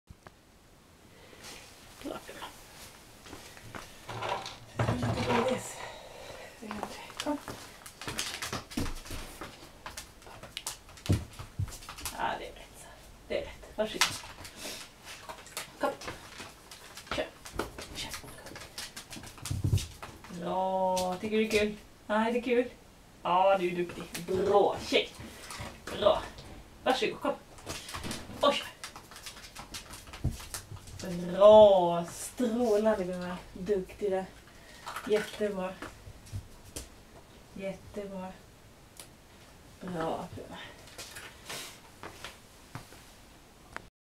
Pumaträning
Puma kommer igång med träningen igen. Jobbar upp musklerna i fronten bland annat.